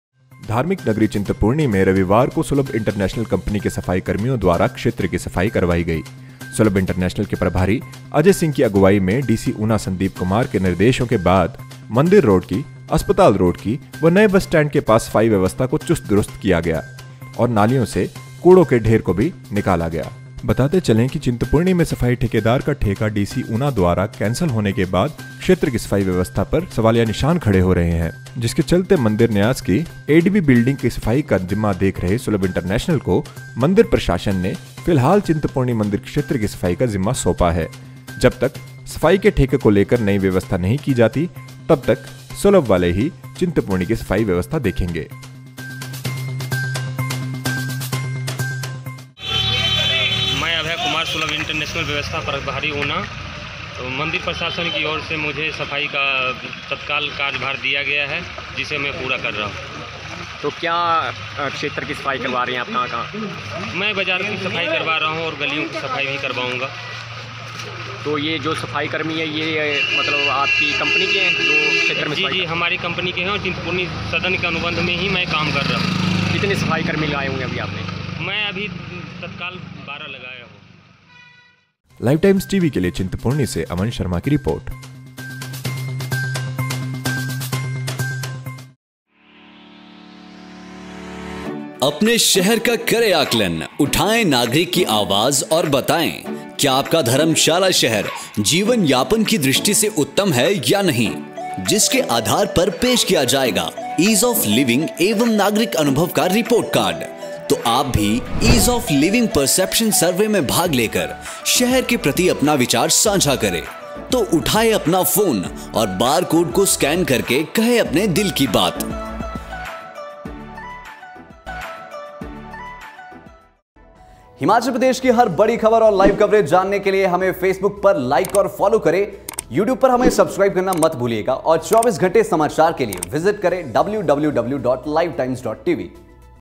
Chintpurni: Sulabh International took out the garbage from the drainage system
चिंतपूर्णी : सुलभ इंटरनेशनल ने सुधारी चिंतपूर्णी की सफाई व्यवस्था नालियों से निकलवाया कूड़ा।